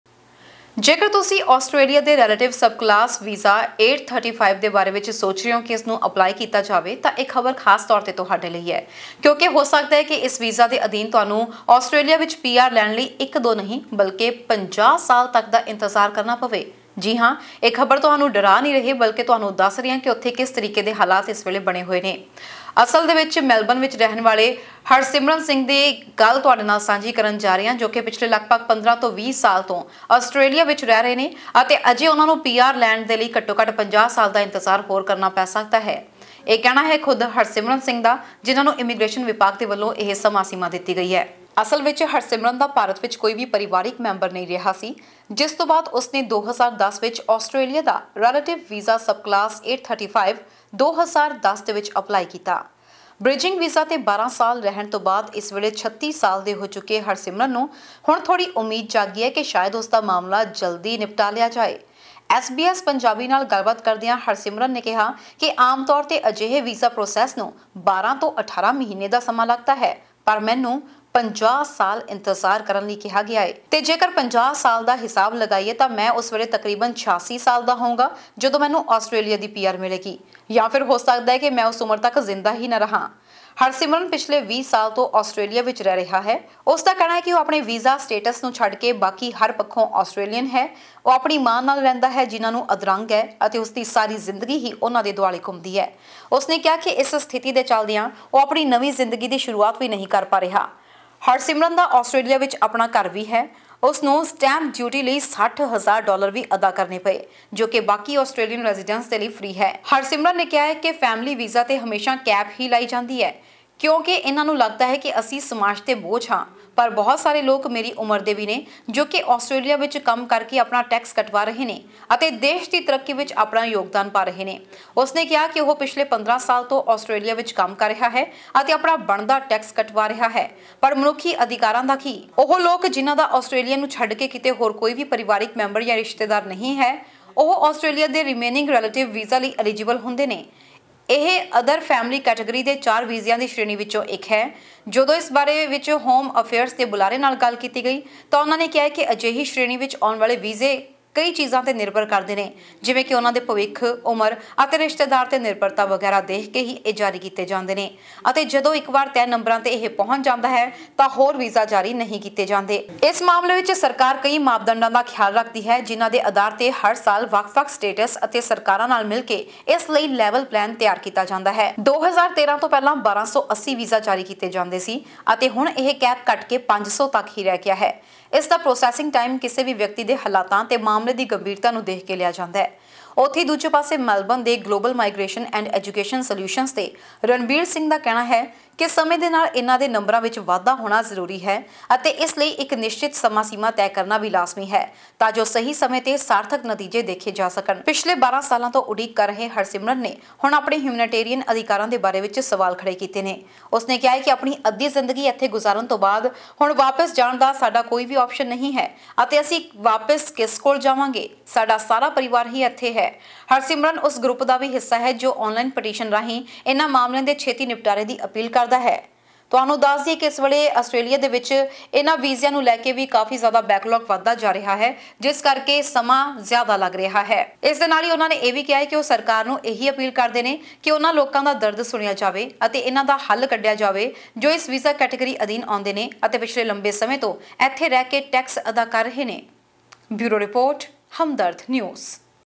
Australia: P.R. ਲਈ ਇਸ Punjabi ਨੂੰ ਕਰਨਾ ਪਏਗਾ 50 ਸਾਲ ਤੱਕ ਇੰਤਜ਼ਾਰ || Hamdard tv
2010 'ਚ ਅਪਲਾਈ ਕੀਤਾ ਸੀ ਰੈਲੇਟਿਵ ਵੀਜ਼ਾ ਸਬਕਲਾਸ 835 'ਚ
ਪਿਛਲੇ 12 ਸਾਲਾਂ ਤੋਂ ਸਟੇਟਸ ਤੇ ਲਟਕ ਰਹੀ ਤਲਵਾਰ